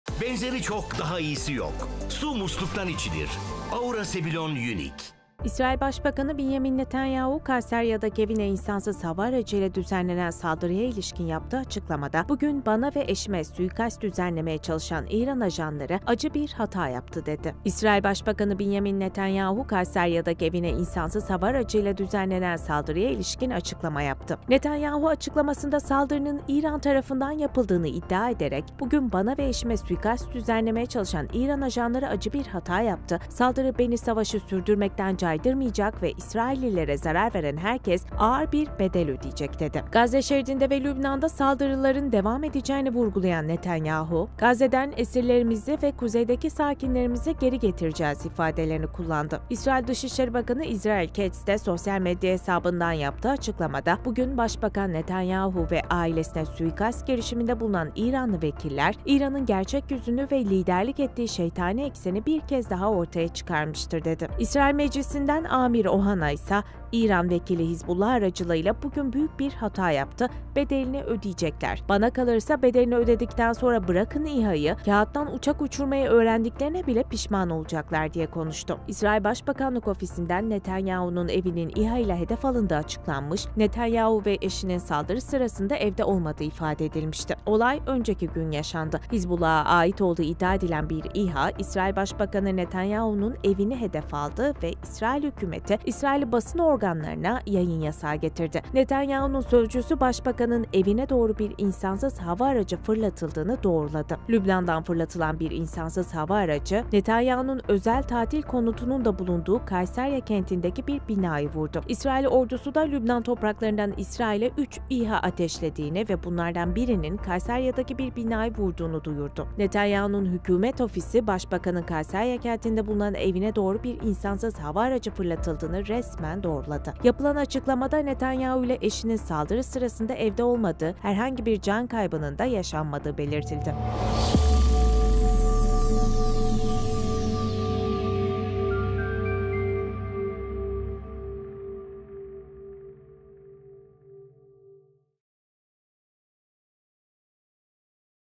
Netanyahu’dan İran’a ‘Suikast’ Tehdidi: Acı Bir Hata Yaptılar Bedeli Ağır Olacak – Türkiye Gazetesi
İsrail Başbakanı Binyamin Netanyahu, Caesarea'daki evine insansız hava aracı (İHA) ile düzenlenen saldırıya ilişkin yaptığı açıklamada, "Bugün bana ve eşime suikast düzenlemeye çalışan İran ajanları acı bir hata yaptı” dedi.

Netanyahu açıklamasında saldırının İran tarafından yapıldığını iddia ederek, “Bugün bana ve eşime suikast düzenlemeye çalışan İran ajanları acı bir hata yaptı. Saldırı beni savaşı sürdürmekten caydırmayacak ve İsraillilere zarar veren herkes ağır bir bedel ödeyecek” dedi.